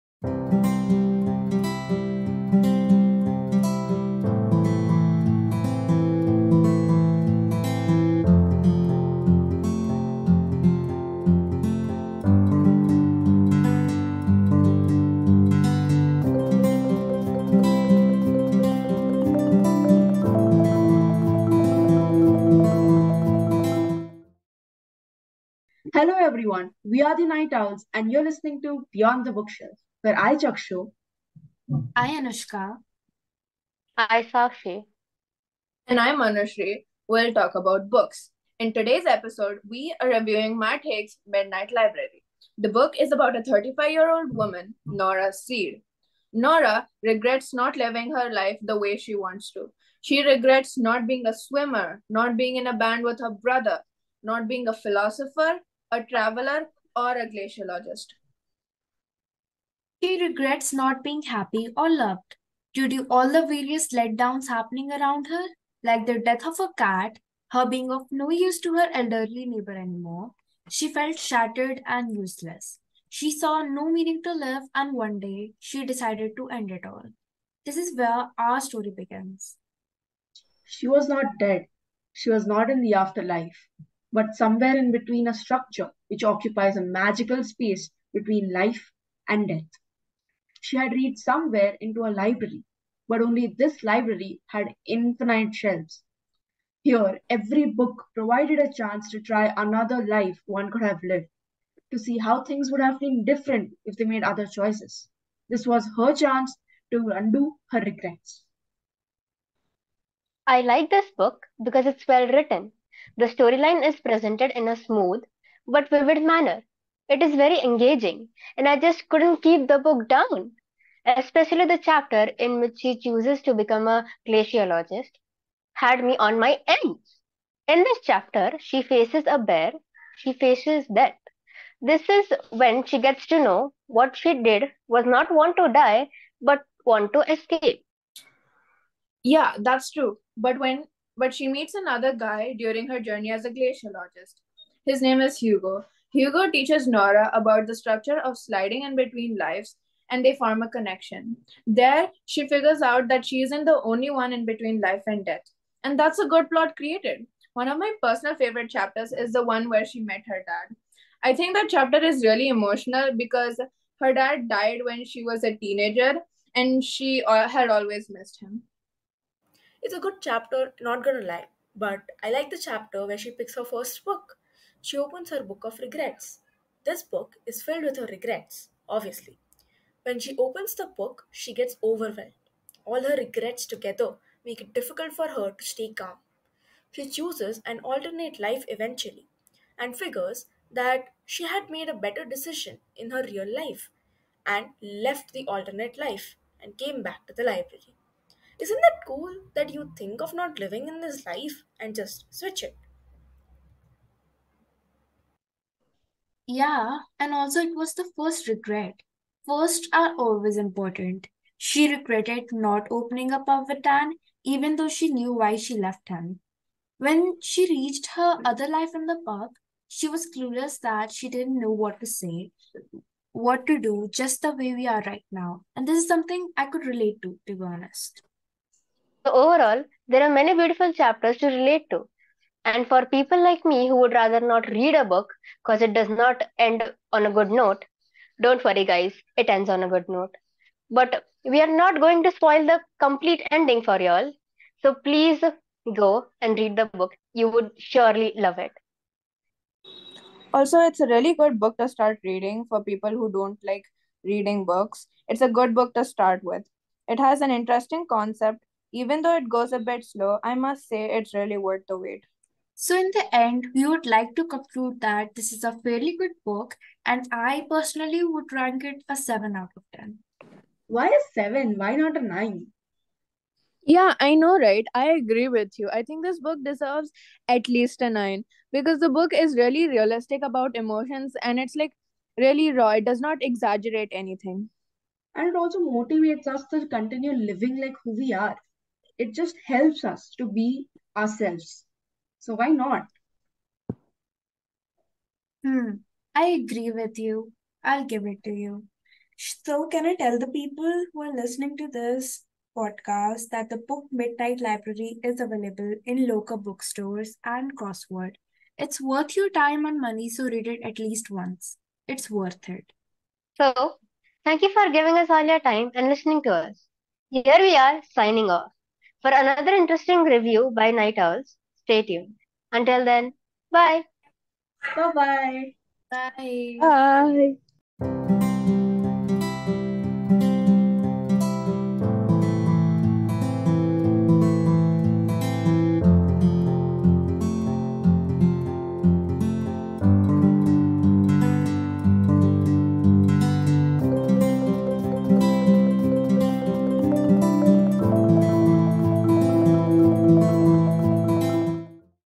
The Midnight Library - Podcast
This podcast is about the literary masterpiece "The Midnight Library" which continues to resonate with readers worldwide. Hear our student's podcast on this masterpiece work.